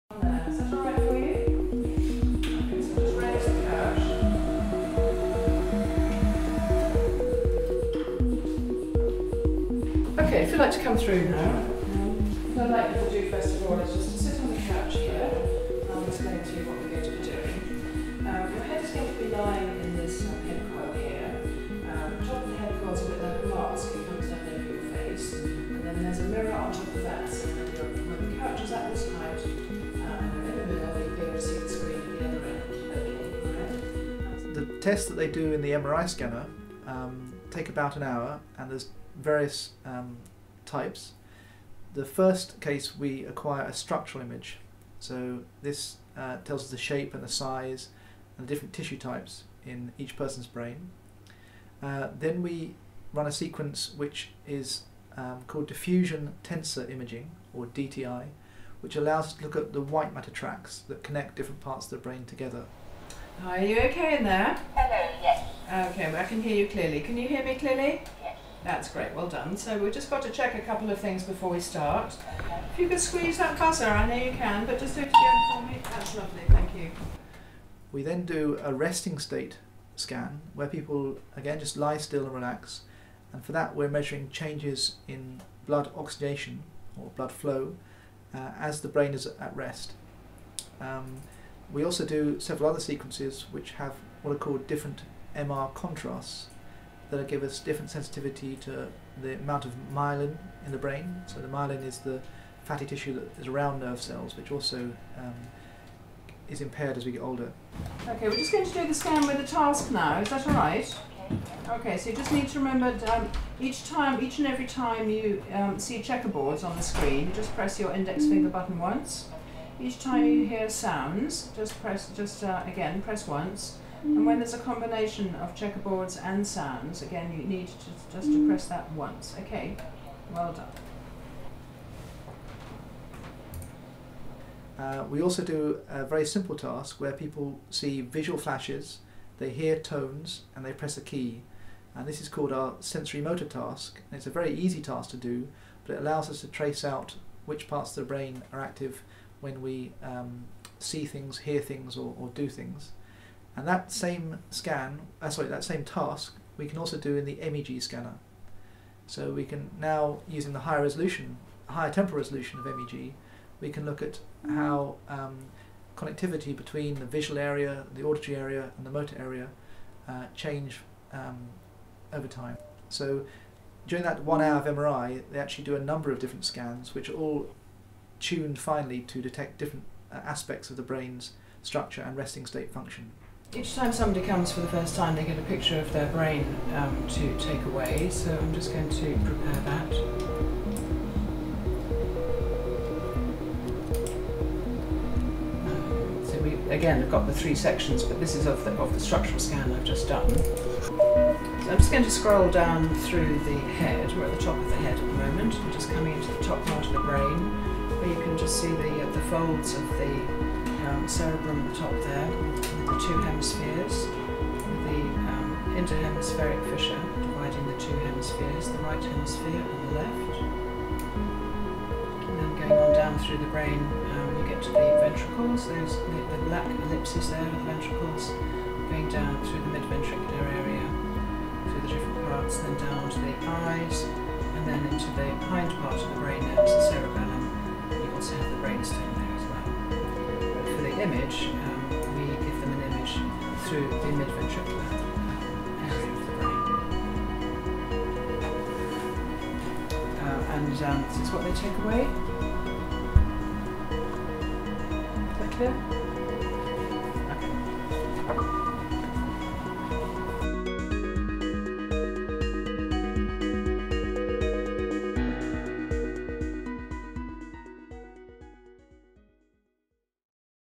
The Power of Advanced BrainImaging Techniques: Insights from the Cam-Can Project
The Cam-Can Project, which focuses on the resilience of the brain, uses advanced brain imaging techniques to gain insights into cognitivefunction. This includes obtaining resting state data and conducting audio-visual tasks on 700 individuals in an MRI scanner, from which 280 are selected for further functional MRI (fMRI) studies. These fMRI studies test activity in various cognitive domains, such as emotion, memory, language, attention, and motorfunction, and are designed by experts in the field.

MRI is considered one of the safest and most accurate brain imaging techniques, as it doesn't involve the risks associated with X-rays or radioactive tracer compounds used in other imaging methods. MRI scanners use powerful electromagnets to measure changes in hydrogen protons in the body as harmless radio waves are applied. Since the human body contains a lot of water, the hydrogen protons are an excellent vehicle to measure soft tissues such as the brain.

Join us in this video as we explore the fascinating world of advanced brain imaging techniques used in the Cam-Can Project. We'll delve into the benefits of using MRI scanners and the insights it provides into the human brain's functioning. This video is perfect for anyone interested in neuroscience, cognitive function, or the latest advancements in brainimaging technology.

Let’s connect!